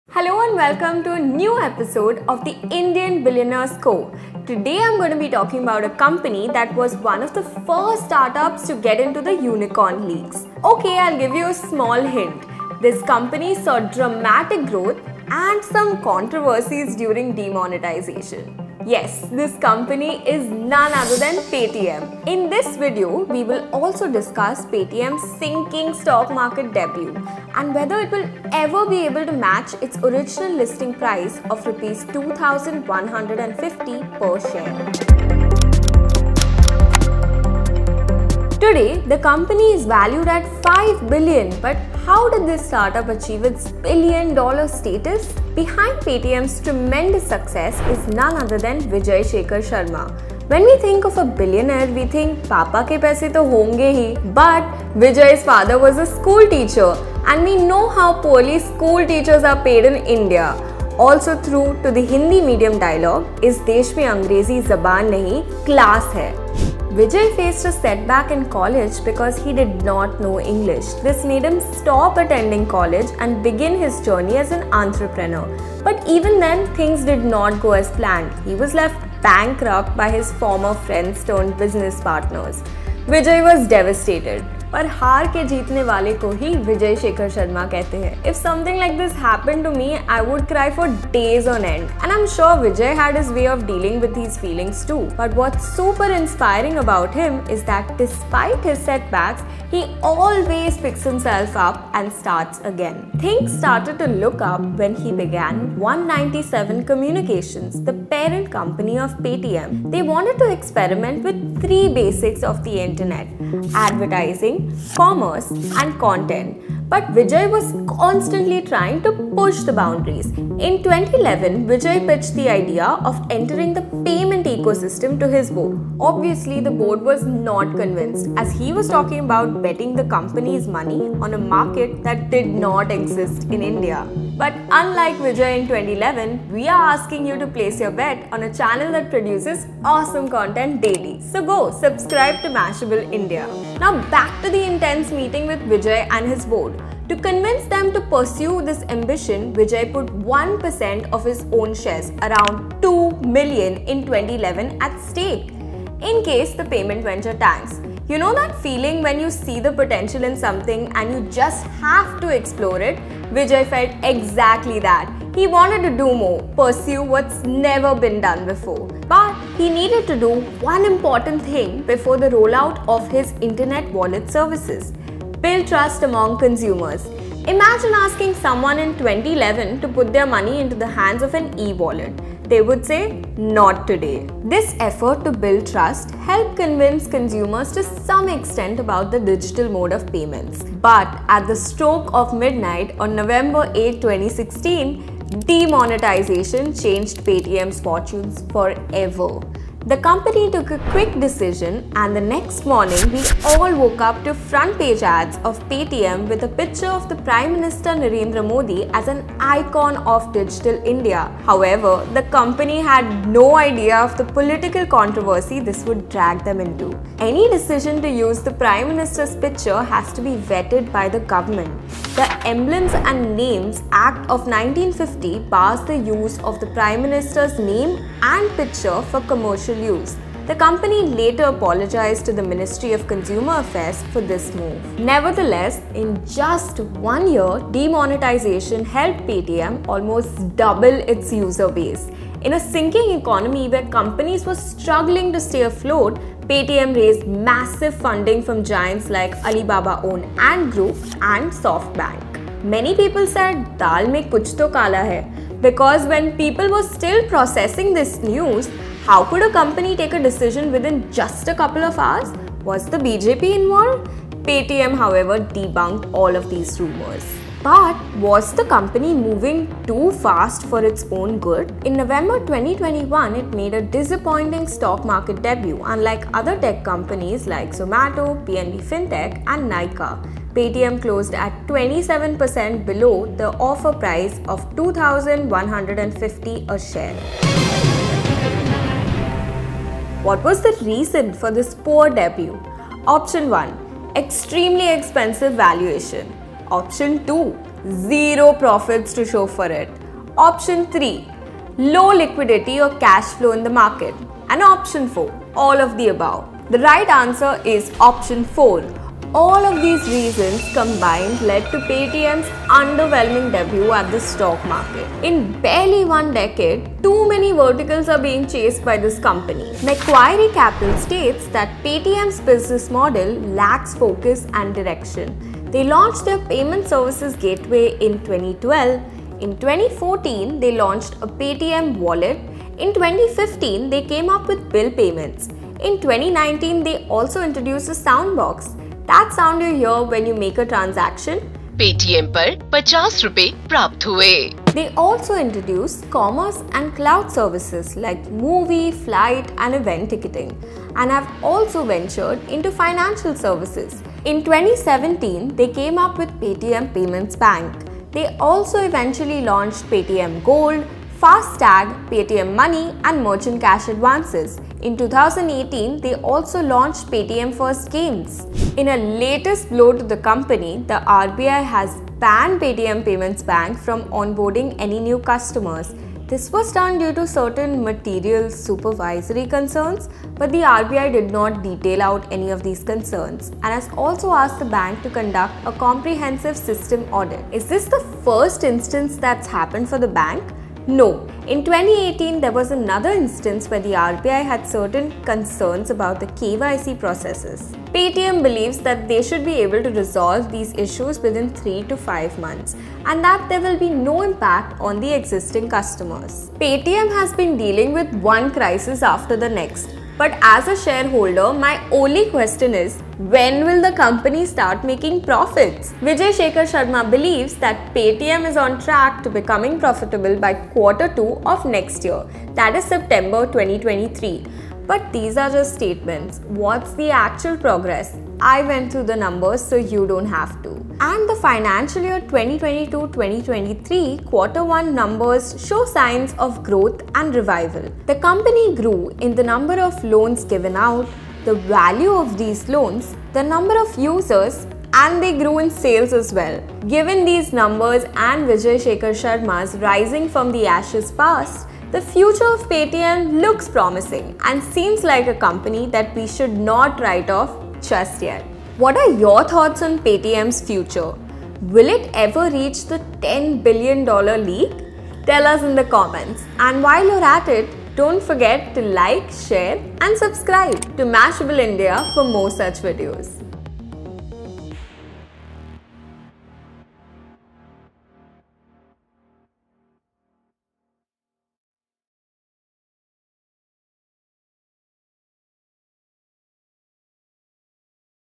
The rise, the fall and potential rise of Paytm | The Indian Billionaires Co. - EP05
Vijay Shekar Sharma’s Paytm has been witnessing one crisis after the next. Will it be able to overcome these challenges and turn profitable by 2023? Watch this video to find out.

0:00 Introduction to Paytm
0:48 Paytm founder - Vijay Shekar Sharma’s journey
3:15 Demonetization impact & controversies
4:32 Why is Paytm sinking?
6:52 Paytm and profits